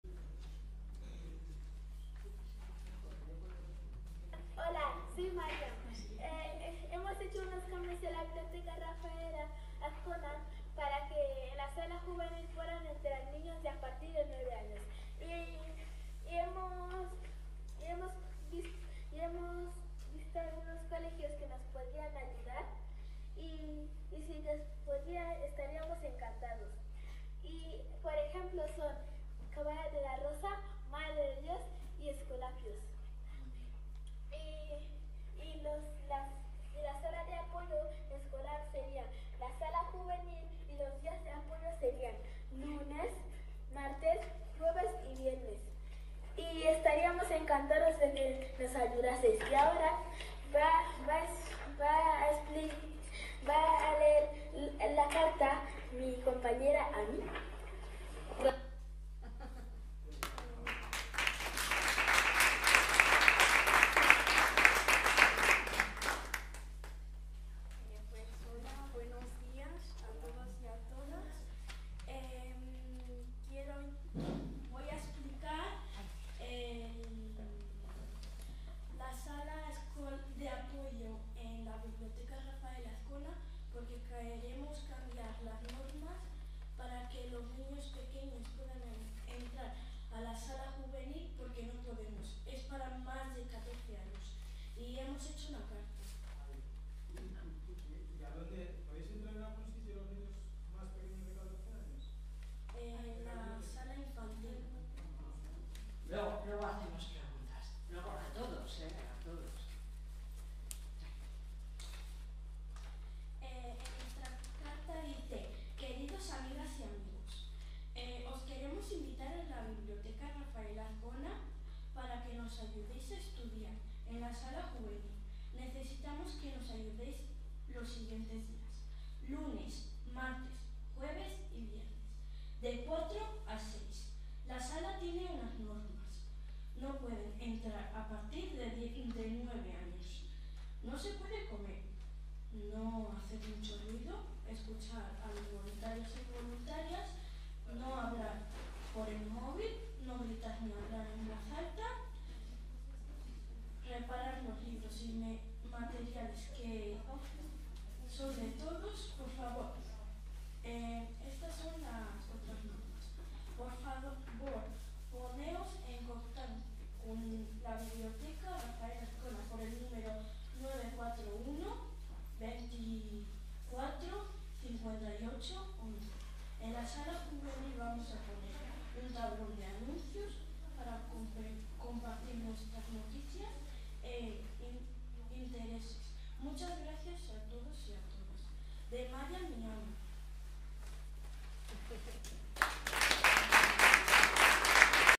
Presentación del proyecto: Aprender actuando
Presentación del proyecto: Aprender actuando del Laboratorio Ciudadano ‘’Entrar-Quedarse, Salir-Quedarse’’ de la BibliotecaRafaelAzcona, @LogroComun, @RiojaAcoge, agendaurbanalogroño y labbbs